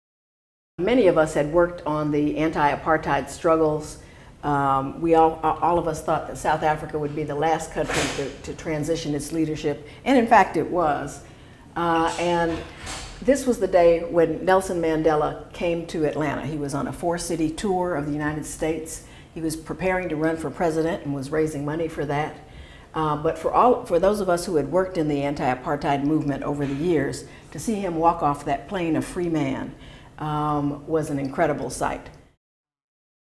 A Phoenix Reborn: Sue Ross #3
Sue Ross discusses Nelson Mandela photo and the anti-apartheid movement.